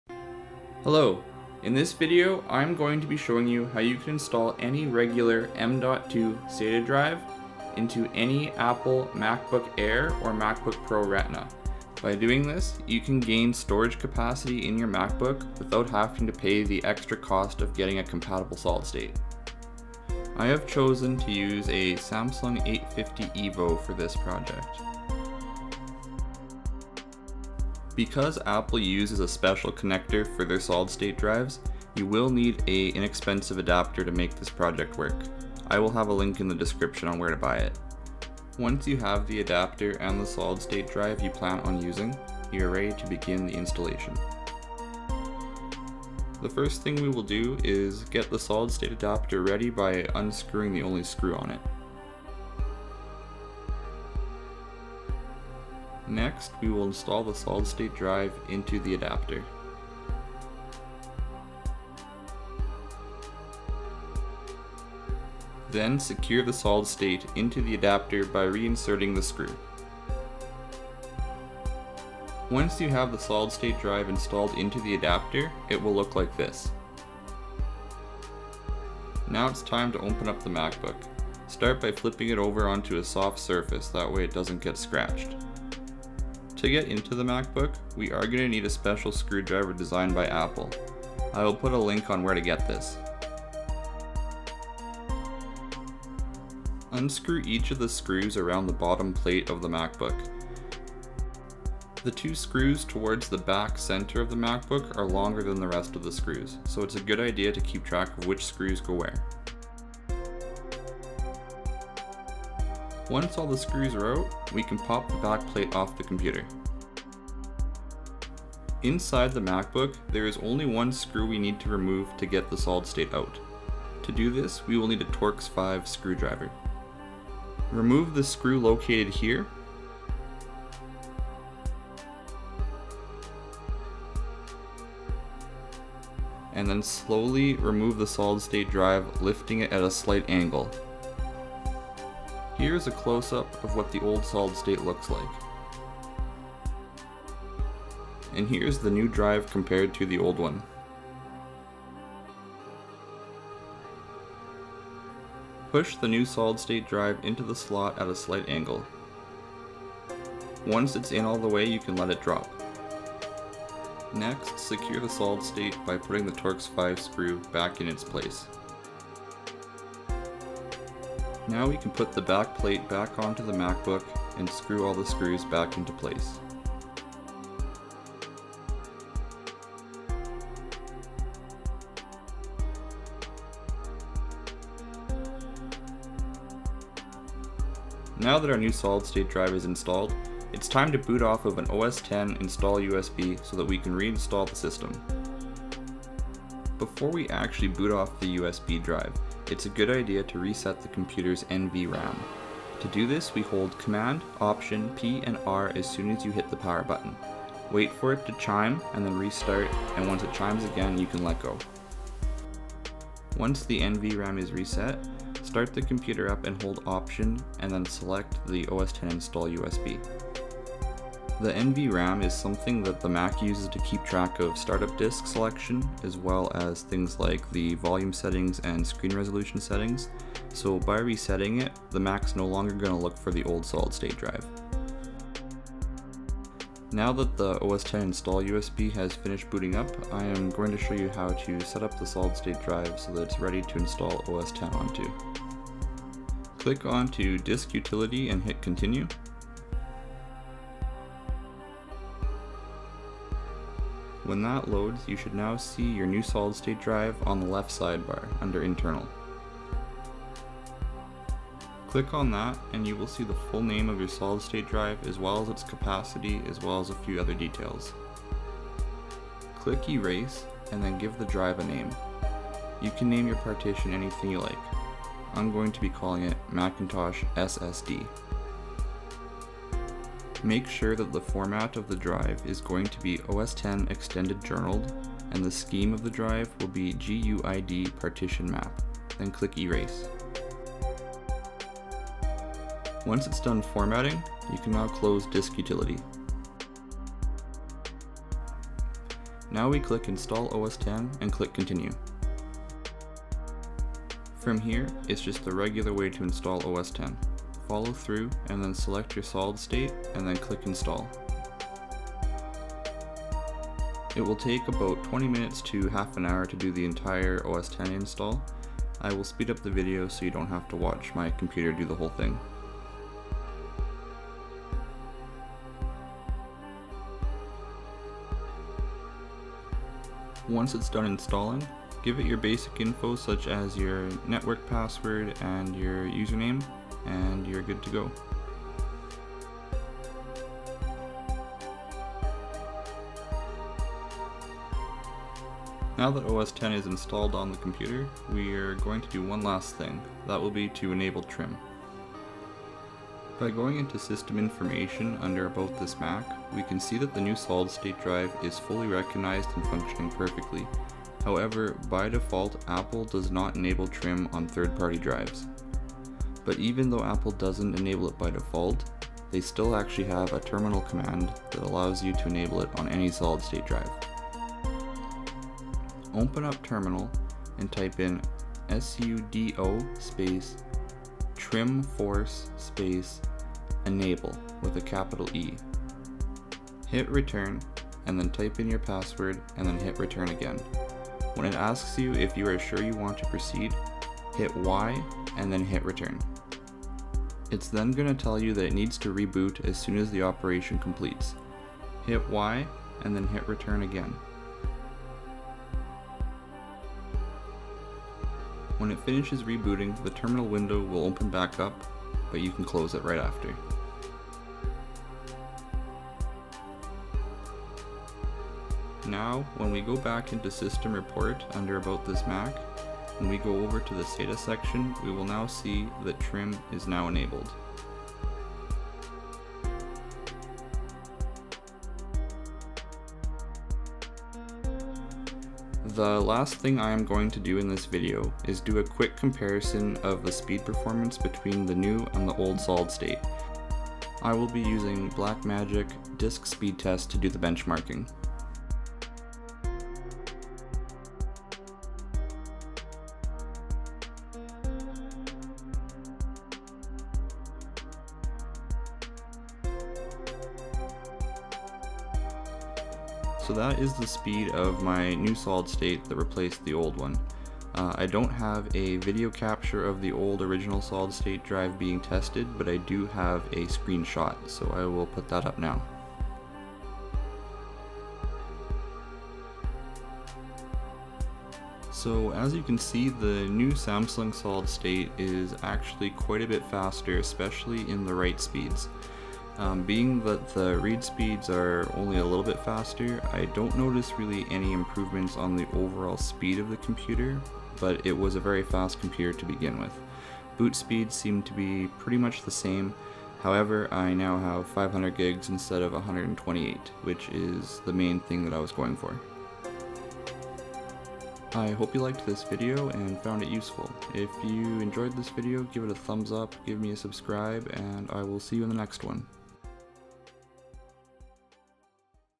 Upgrade Your Macbook Air SSD with Any M.2 SATA Drive
In this video I will be showing you how to replace the storage drive in your MacBook Air or Pro Retina for much cheaper by using any regular M.2 SATA Solid State Drive. This Will only work on Macbook Air and Pro Retina models that use the SATA III interface for storage.

Adapters:

Macbook Air

2012 M.2 adapter

2012 mSata adapter

Solid State Drives:

Samsung 850 Evo M.2

Samsung 850 Evo mSata (To be used in MacBook Pro Retina with the linked mSata adapter)

For MacBook Air and Pro Retina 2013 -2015:

Note: I have not yet personally tried the adapter for the PCie models as they are quite new from 2017. I will try the adapter and make a video when I do, but I have been messaged by a few users saying they tried it and it worked.

Or if you have a newer MacBook you can go with these option that I know work for sure.

Screwdrivers:

Apple Pentalobe P5

Torx T5

Screwdriver Set